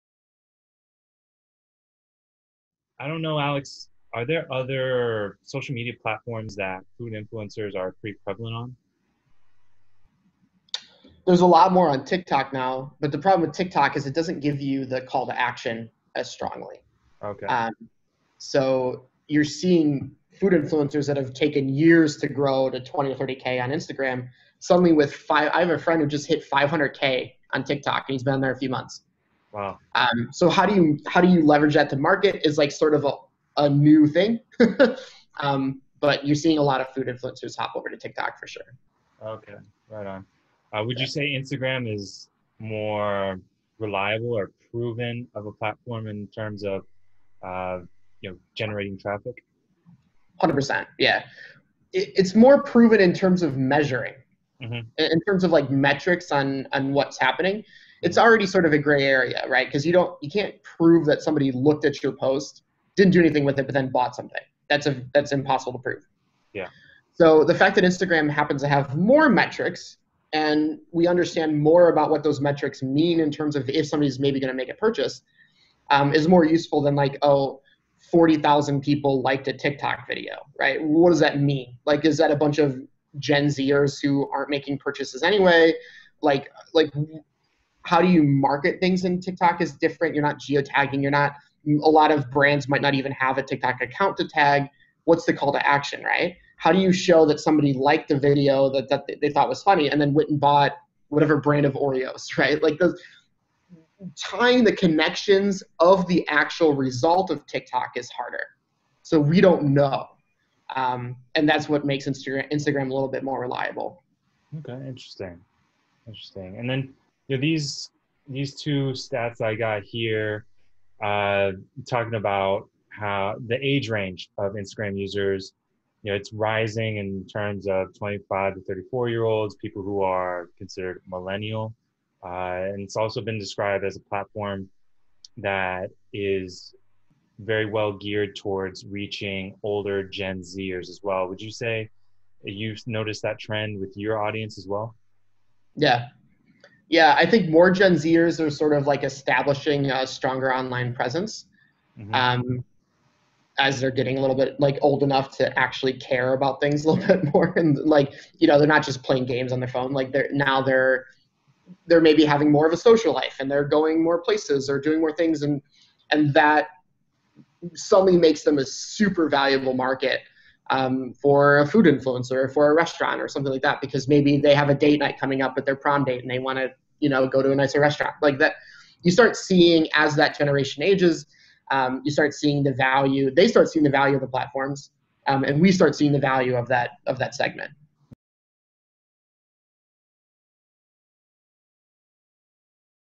Instagram vs. Tik Tok - Which is Better for Restaurants?
@bestfoodalex details his experience with Instagram and Tik Tok for food influencing, and breaks down the advantages and disadvantages for each when it comes to promoting your restaurant.